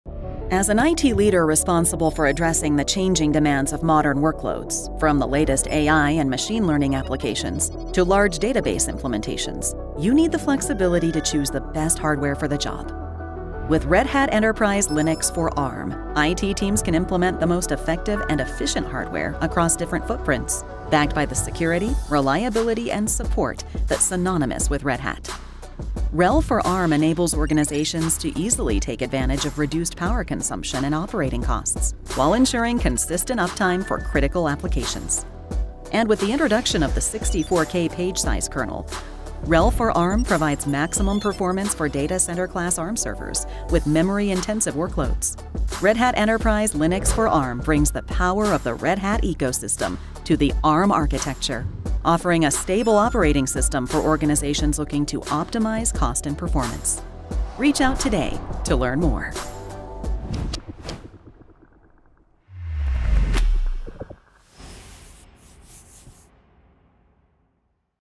Red Hat Enterprise Linux for ARM
Red Hat Enterprise Linux Server for ARM is a part of a Red Hat strategy to support multiple architectures, including x86, IBM POWER, and Z. Red Hat Enterprise Linux for ARM delivers a high-performance, reliable, and more security-focused platform, including a consistent application environment across physical, virtual and cloud deployments.